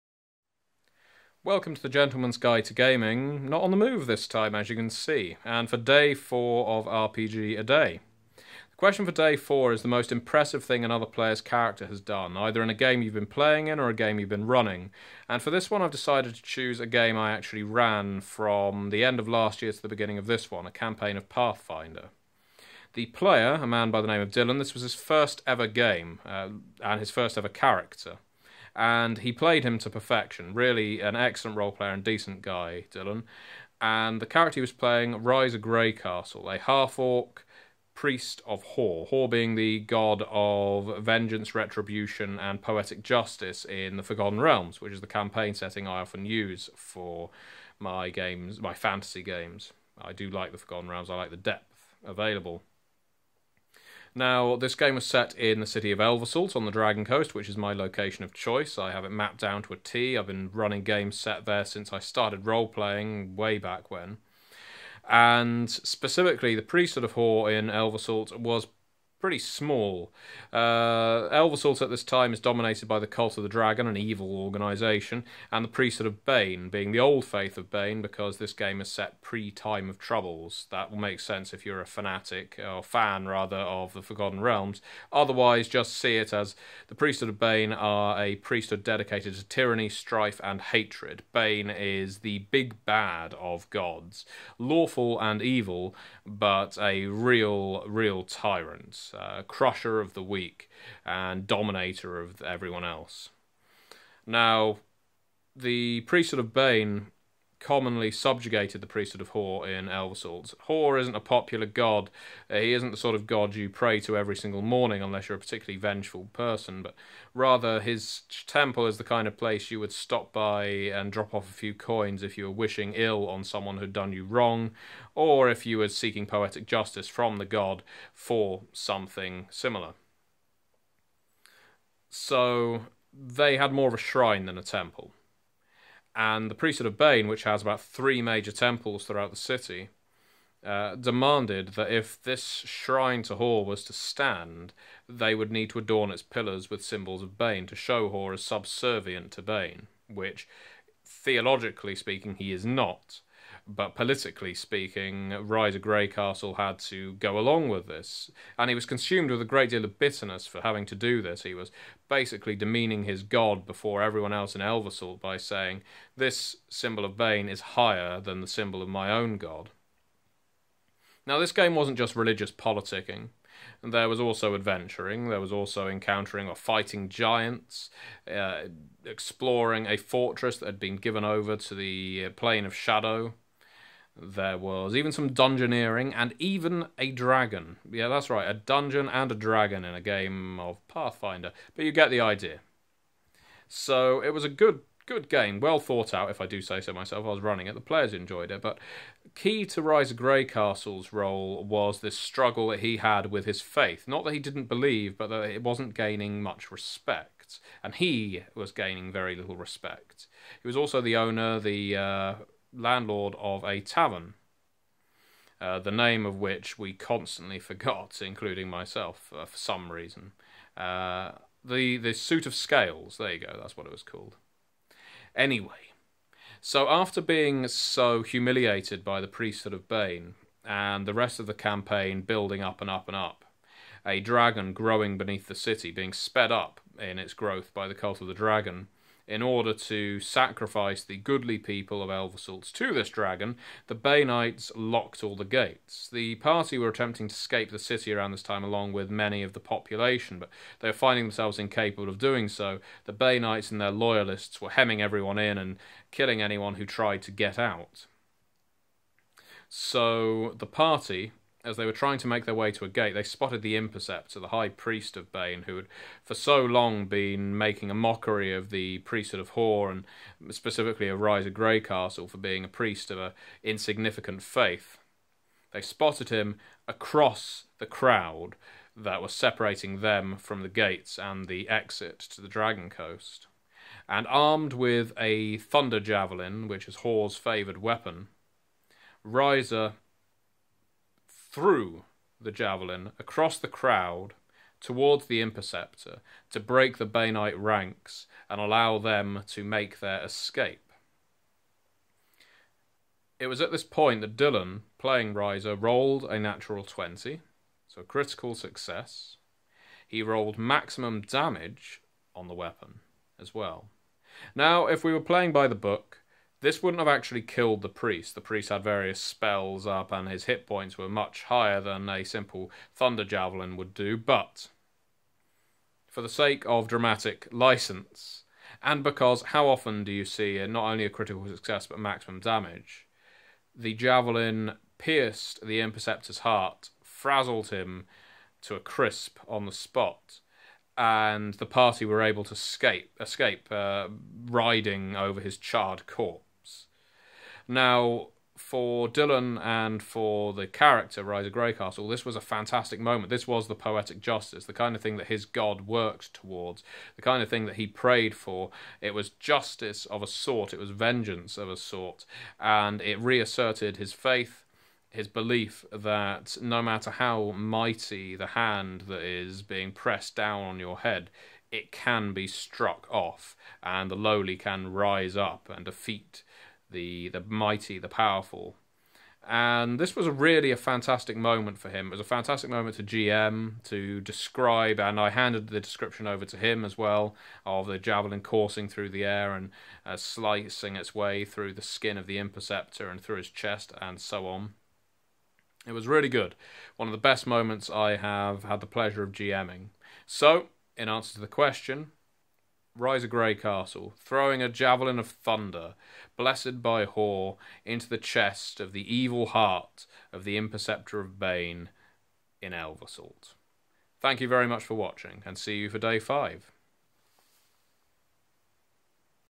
RPGaDay - Day Four - Impressed by other Players - Pathfinder RPG
In which the gentleman in question recaps the misadventures of one Riser Greycastle, a half-orc priest in a campaign of Pathfinder set in the Forgotten Realms.